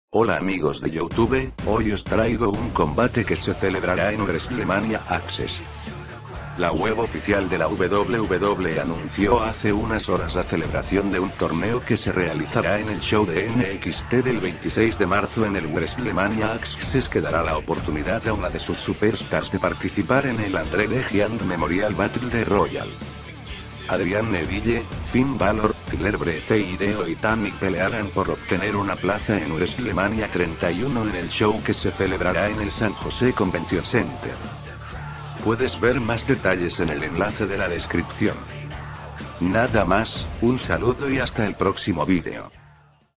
INFORMACIÓN NXT | LUCHADOR DE NXT EN WRESTLEMANIA 31
ADRIAN NEVILLE, FINN BÁLOR, TYLER BREEZE Y HIDEO ITAMI PELEARAN POR OBTENER UNA PLAZA EN WRESTLEMANIA 31
El ganador del torneo que se celebrará el 26 de marzo en el Wrestlemania Axxes podrá participar en el Andre the Giant Memorial Battle Royal.

Un Saludo.